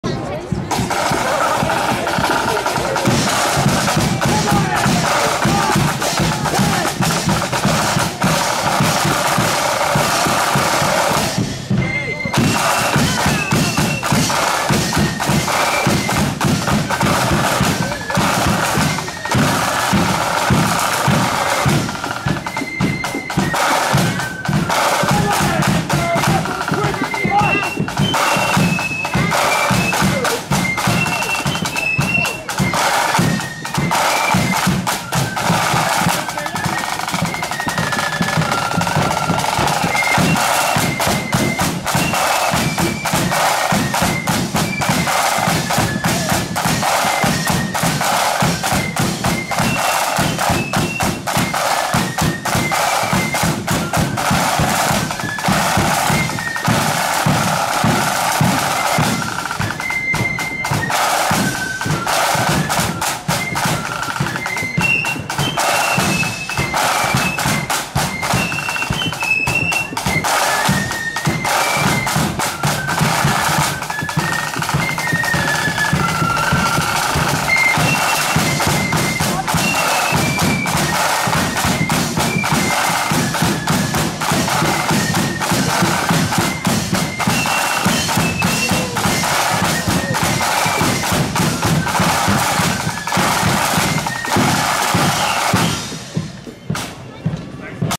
Ulster Grenadiers (No.1) @ East Antrim Combine 12th ~ Antrim ~ 12/07/22 (4K)
Ulster Grenadiers Flute Band parading through the streets of Antrim at the East Antrim Combine annual 12th parade held on Tuesday 12/07/22.